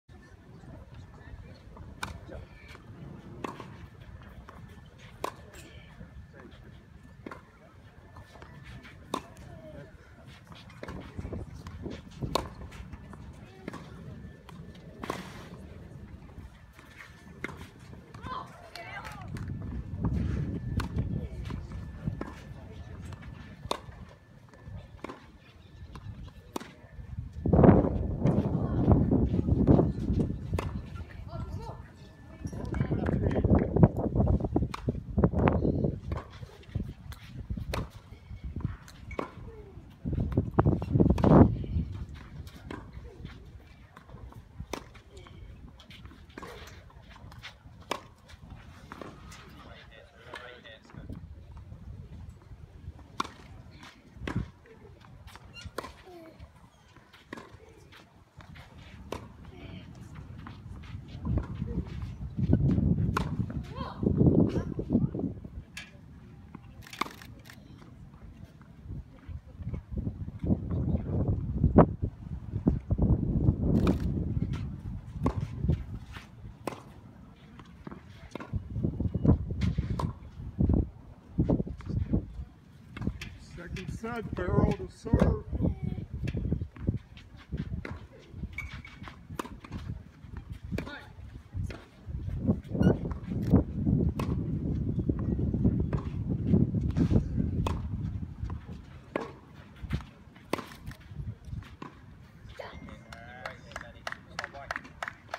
Benjamin Gusic Wan 2019 JOB 12s final
Benjamin Gusic Wan in Junior Orange Bowl B12s final, December 22, 2019, Salvadore Park, Coral Gables Florida. Gusic Wan defeated Alejandro Arcila 6-3, 6-3.